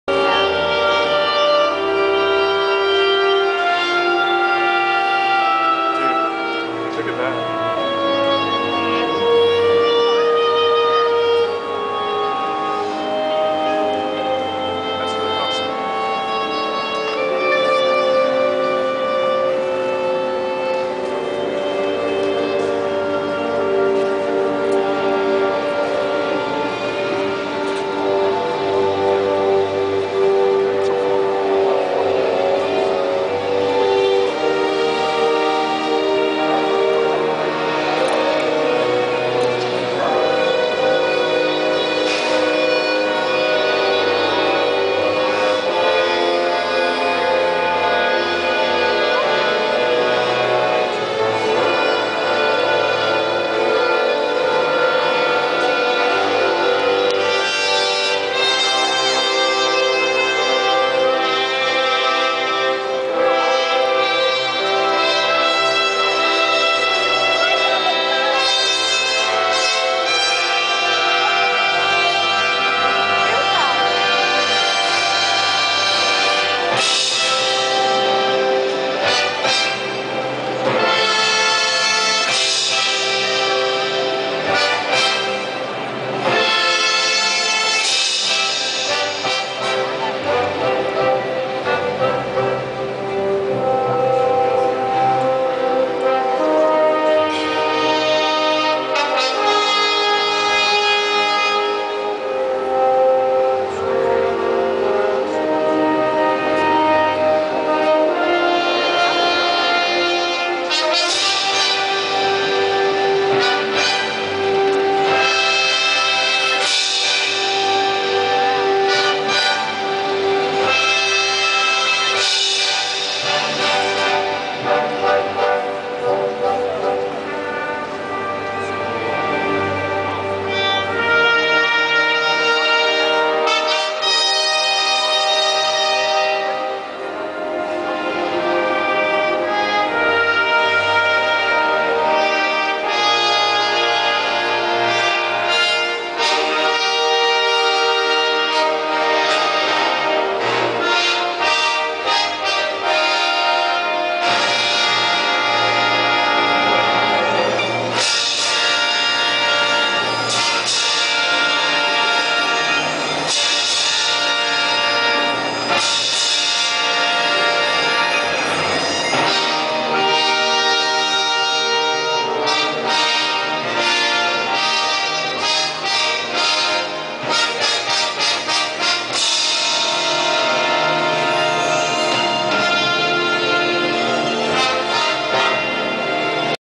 Rhode Island Philharmonic at Waterfire August 2010 IV
Providence, 2010. anyone know the piece? I think this was either the Mahler or Puccini, but not positive